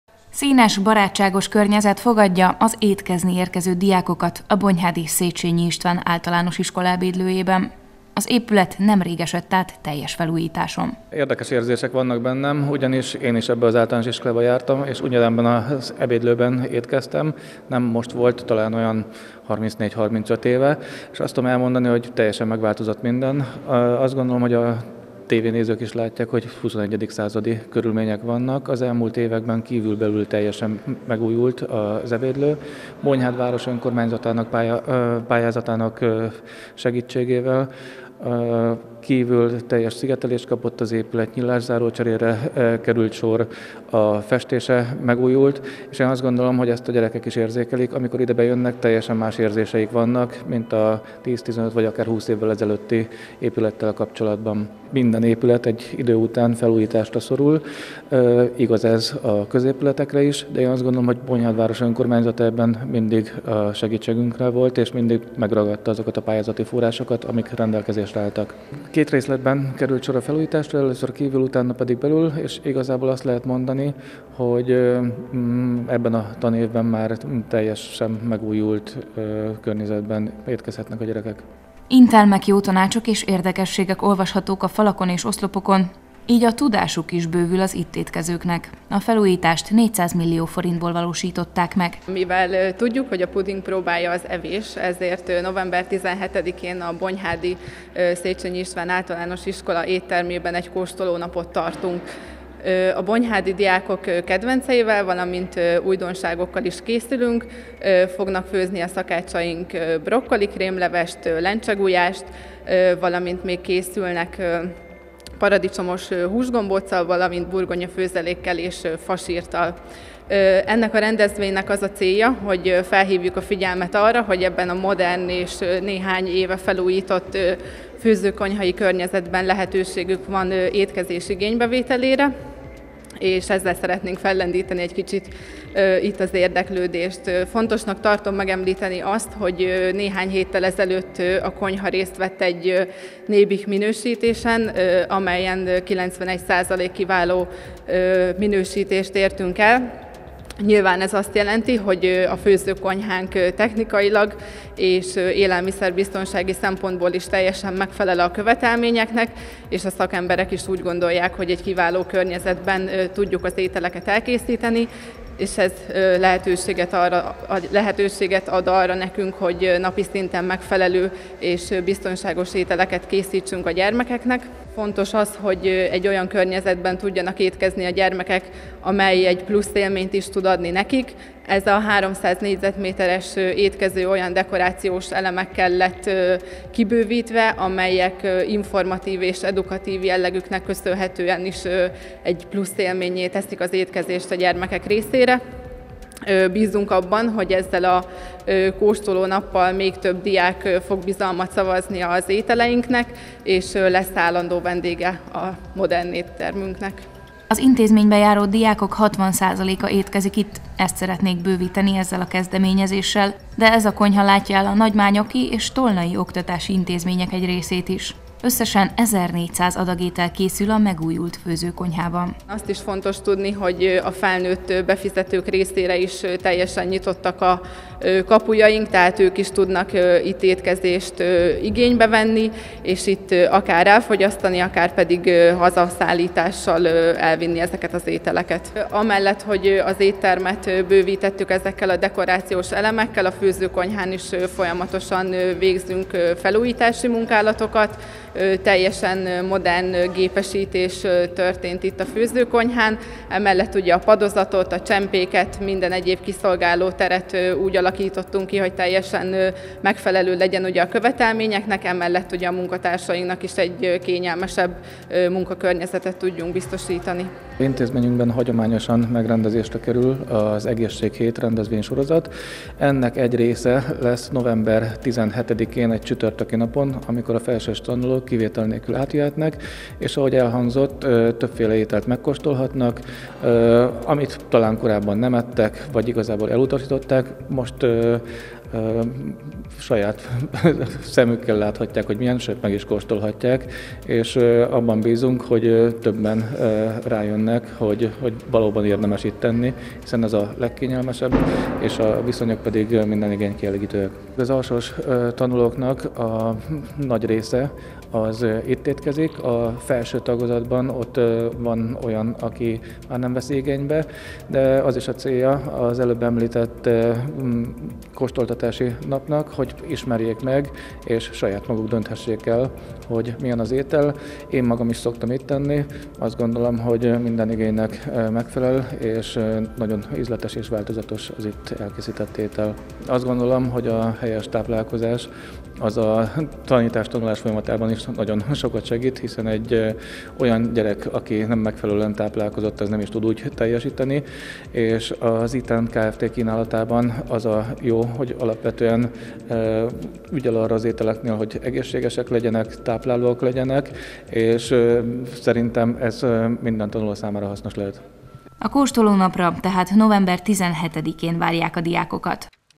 Kóstoló napra készülnek a bonyhádi Széchenyi iskola ebédlőjében
Kóstoló napra készülnek a bonyhádi Széchenyi István általános iskolához tartozó ebédlőben, hogy bemutassák a diákoknak, koránt sem olyan rossz dolog menzásnak lenni. A felújított és modernizált ebédlőben az intézmény diákjainak 60 százaléka étkezik, viszont a főzőkonyha 1400 adag ételt készít minden nap.